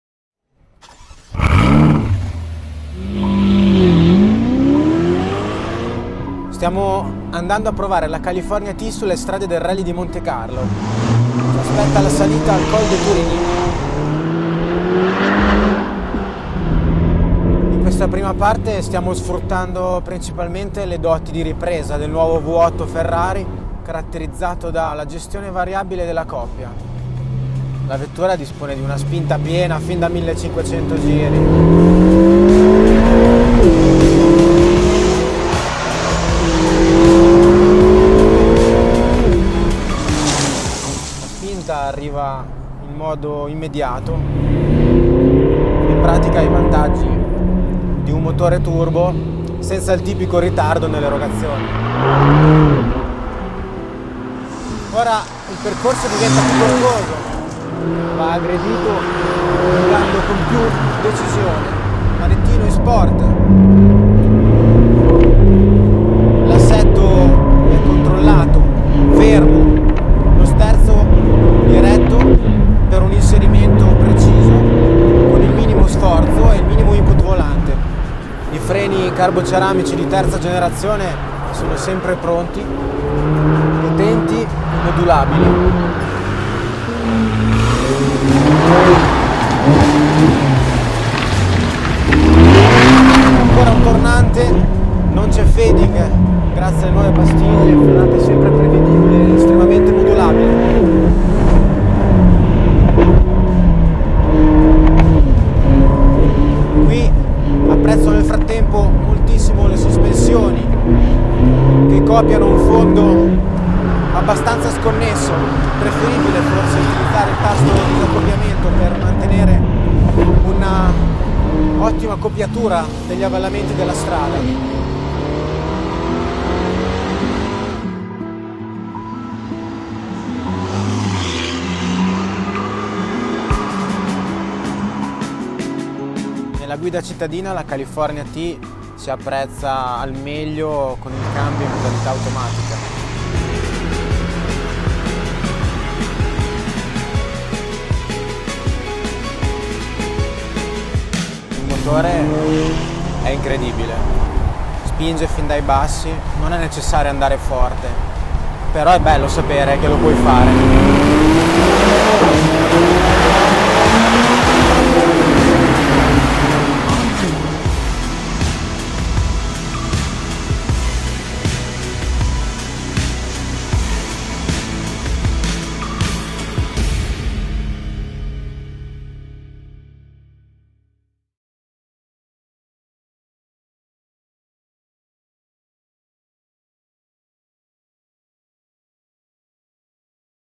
Ferrari California T takes on Monte Carlo
פרארי קליפורניה T במונטה קרלו - רפאל דה-סימון, ראש מחלקת פיתוח נהגי מבחן מאחורי ההגה.

Ferrari’s head of development test-drivers Raffaele De Simone driving the California T at the challenging streets and roads of the Principality of Monaco